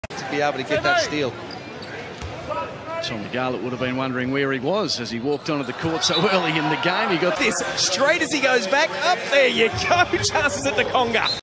When You Shoot A Free Throw And It Goes Behind You… 🤦‍♂️
Footage from Highlight Heaven